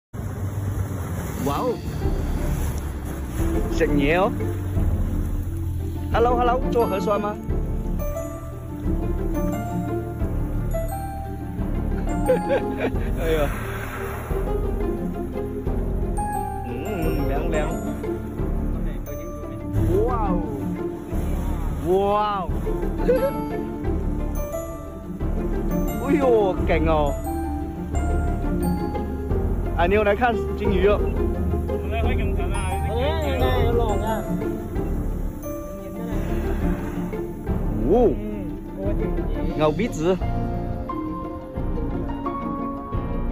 Flaud Kong Viral WTF Is That At Sea Videos
@flaudkongviraltrender wanted to share rhis video we thought was amazing. hope you enjoy these whales feeding on top the ocean. some of us will never get to see things like this. youtube shortslist, video2023 whales @whales